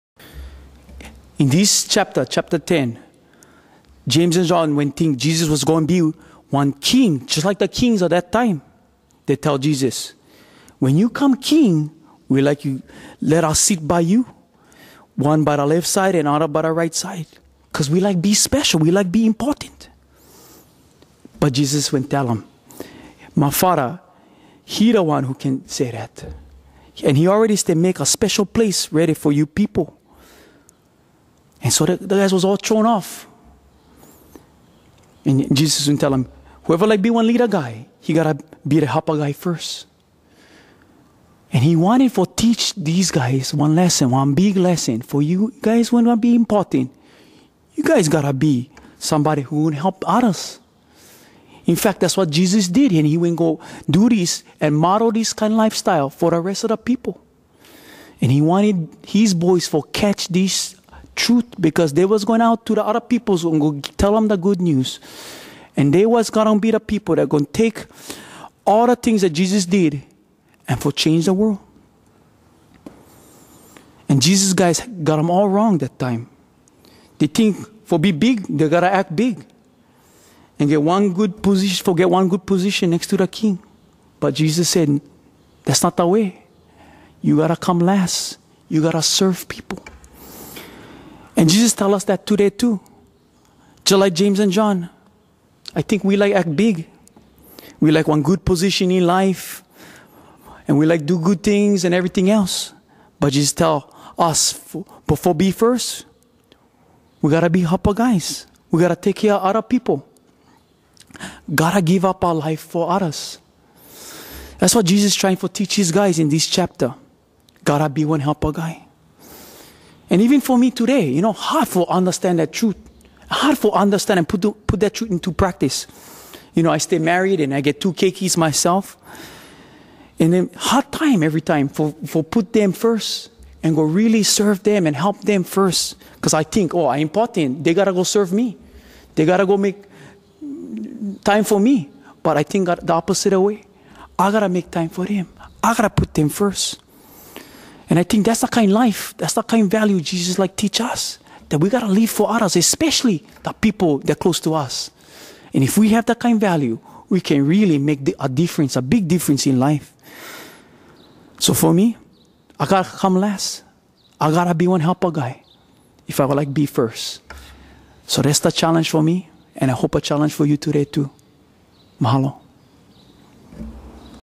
Da Hawaii Pidgin Bible, Homily for Mark Chapter 10
Earl Morihara, closes with a homily after reading Chapter 10 of the Book of Mark from Da Jesus Book